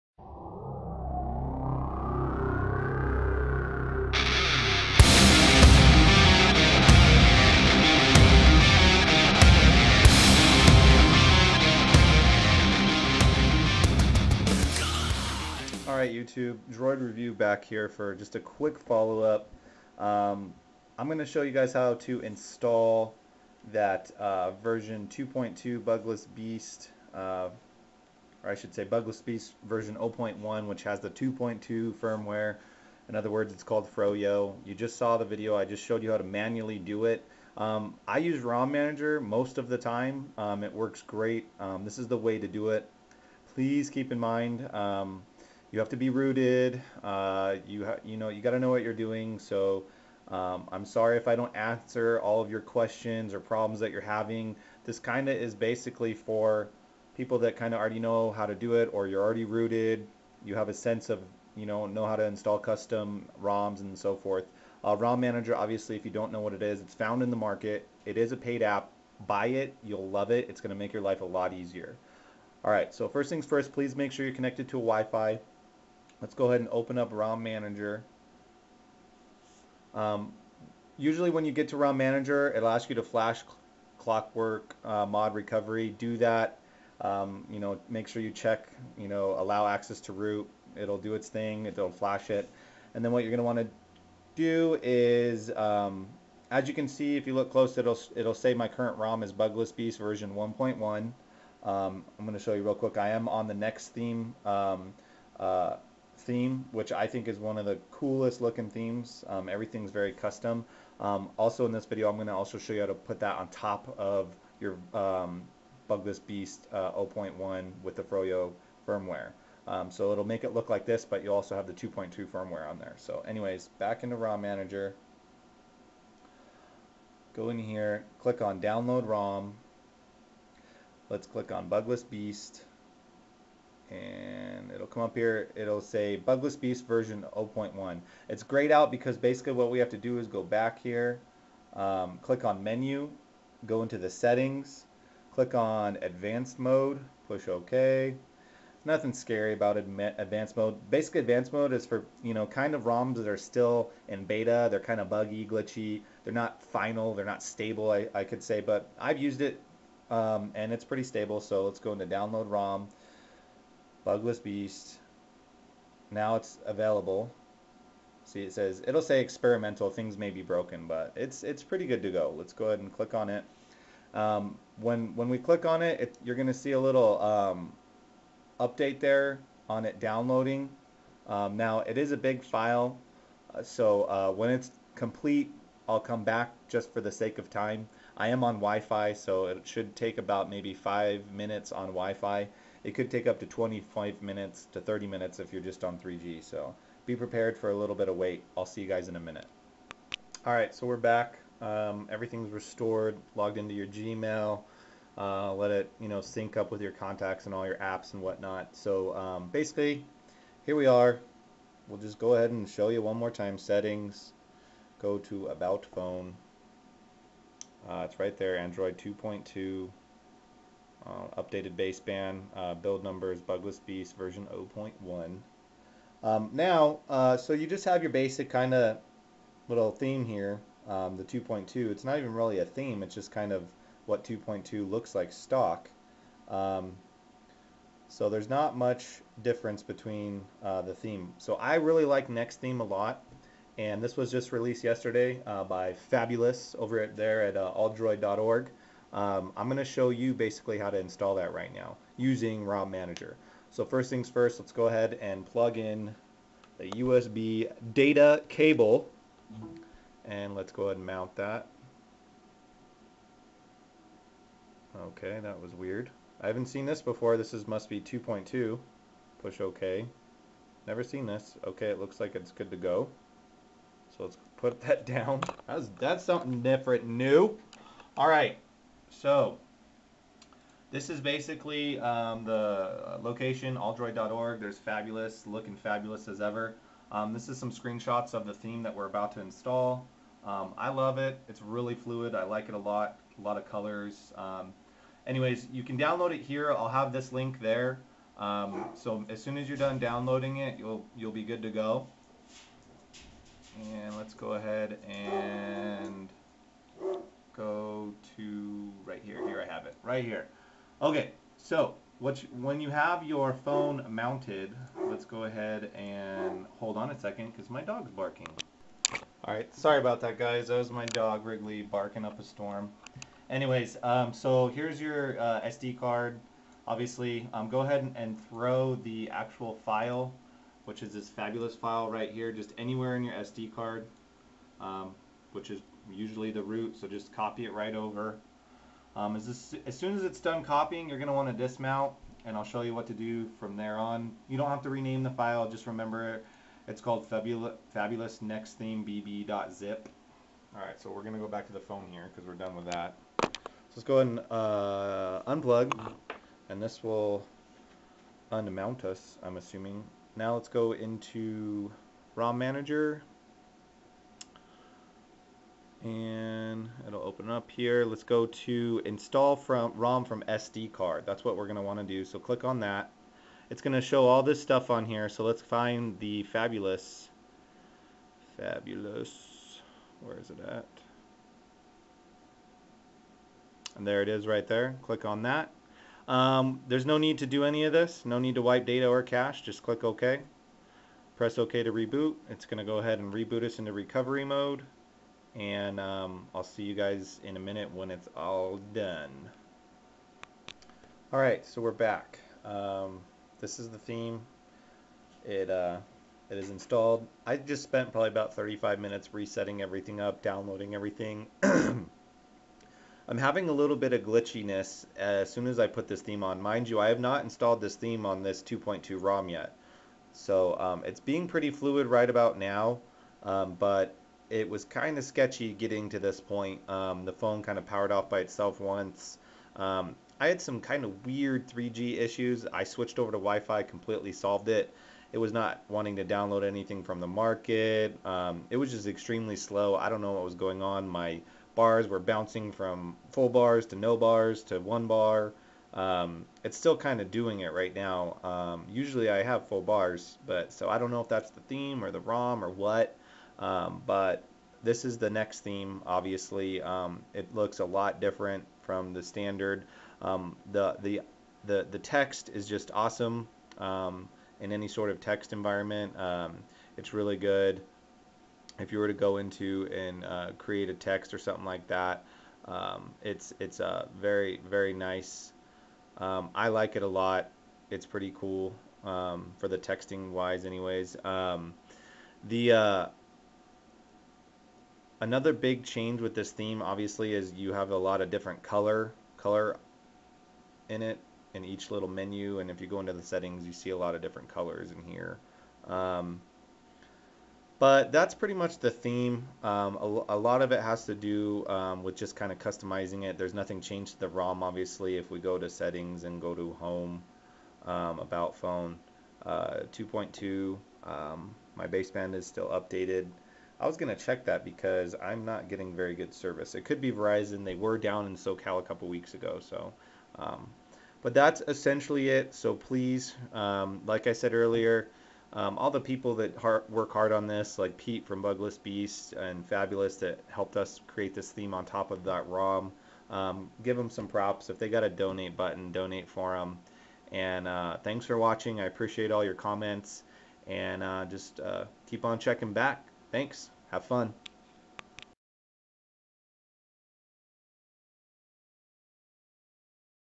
Motorola Droid - How to install 2.2 w/NexTheme using Rom Manager!
Here is just a quick follow up to my last video. BUT this way is using Rom Manager to install it, much much easier.

Also gives you the installation of the NexTheme via Rom Manager too!

Make sure you follow the video very carefully, again this is for experienced ROOTED users only!

-DroidReview